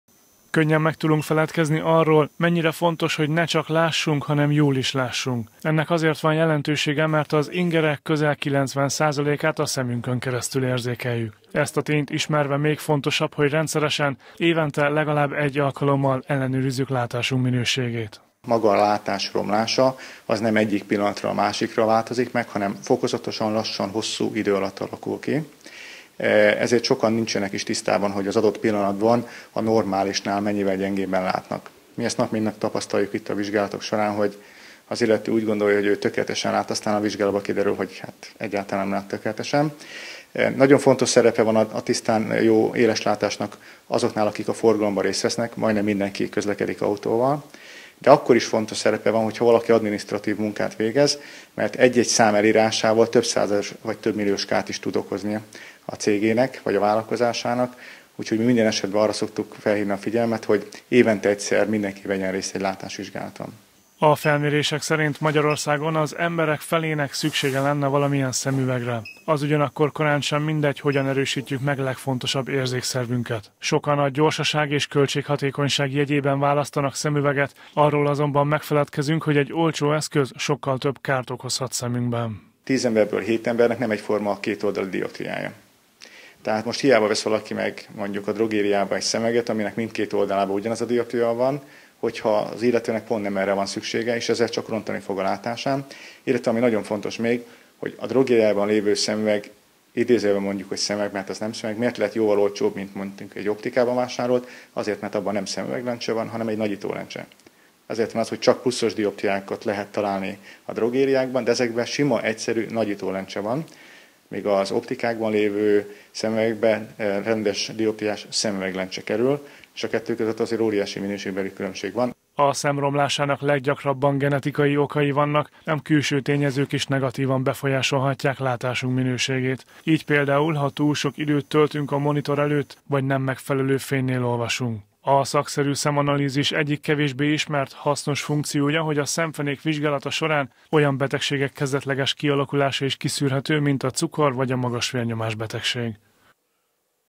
Fontos a rendszeres szemvizsgálat - Kölcsey Televízió
Magyarország lakosságának közel fele rendelkezik valamilyen fokú látásproblémával. Noha köztudott, a szem a legfontosabb érzékszervünk, sokan mégsem fordítanak rá elég figyelmet. Szintén nagy azoknak a száma, akik kihagyják a szakszerű szemvizsgálatot és olcsó szemüveget vásárolnak, ez azonban még több kárt okozhat.